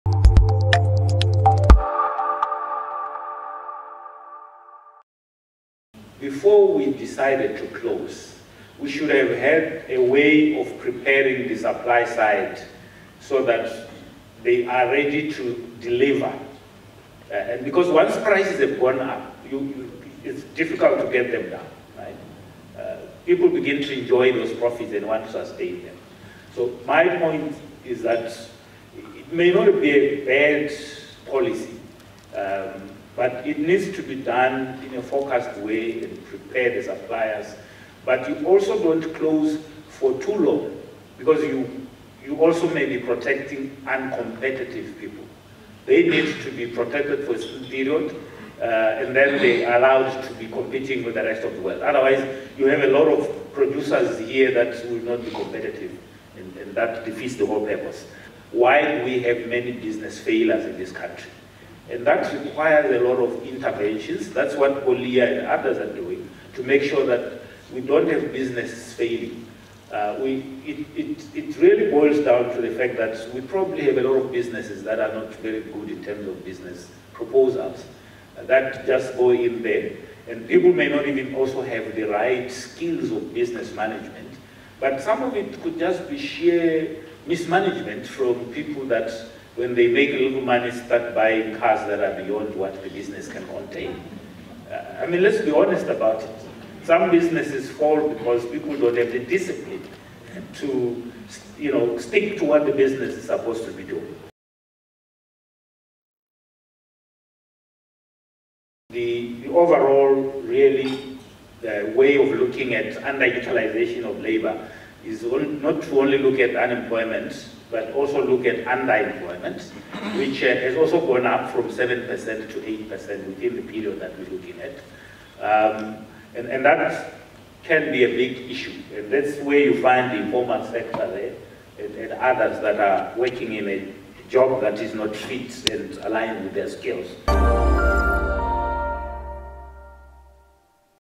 Expert remarks on the ban of Horticultural Imports
Almost two years after a ban was imposed on imports of horticultural products, the debate is still raging on whether its implementation was properly handled. Professor Happy Siphambe (UB) recently made his contribution at a Labour Conference organized by the Institute for Labour and Employment Studies.